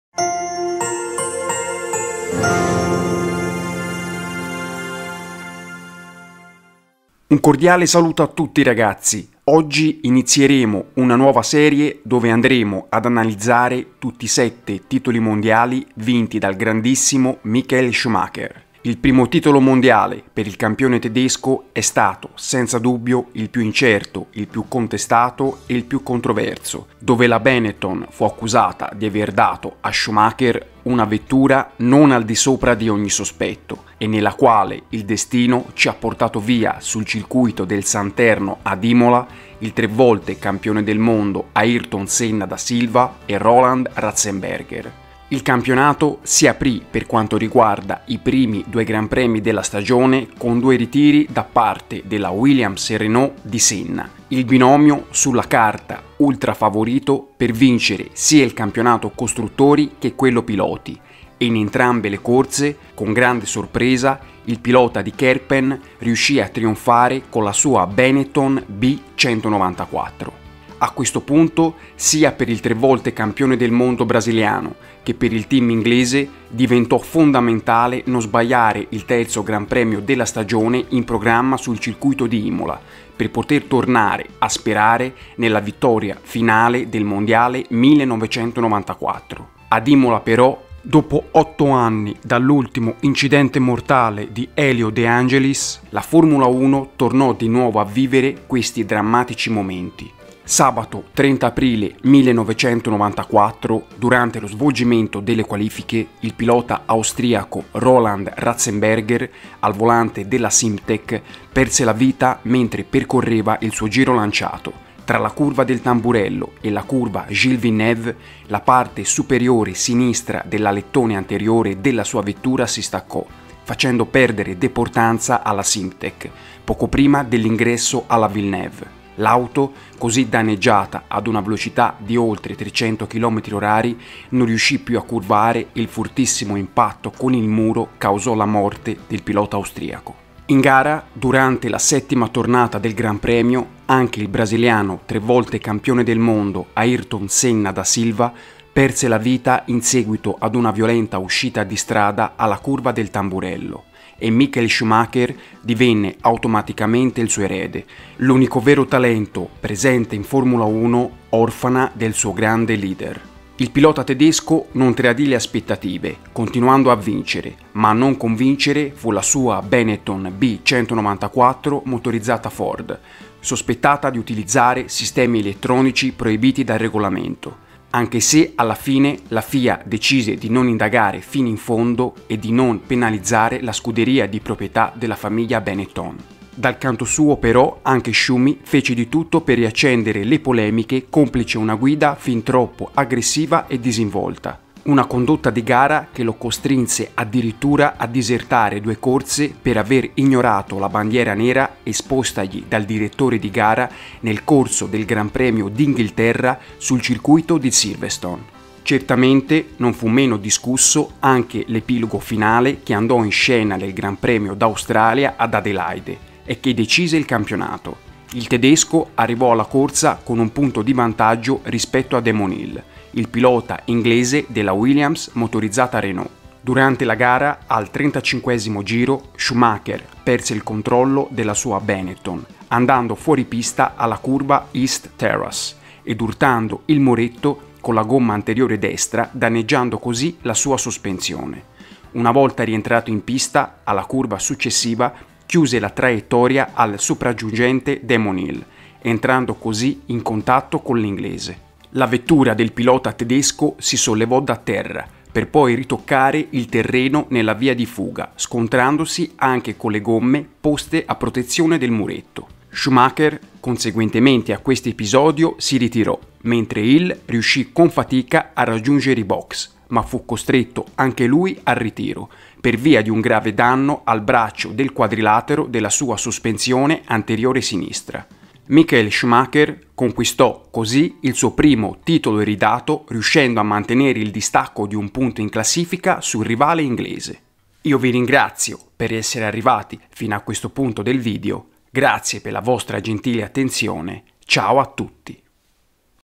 Michael Schumacher | Il 1° Mondiale di Schumy al volante della Benetton B194 - 1994
Oggi inizieremo una nuova serie dove andremo ad analizzare tutti i 7 titoli mondiali vinti dal grandissimo Michael Schumacher. Il primo titolo mondiale per il Campione tedesco è stato senza dubbio il più incerto, il più contestato e il più controverso dove la Benetton fu accusata di aver dato a Schumacher una vettura non al di sopra di ogni sospetto e nella quale il destino ci ha portato via sul circuito del Santerno ad Imola, il tre volte Campione del Mondo Ayrton Senna da Silva e Roland Ratzenberger.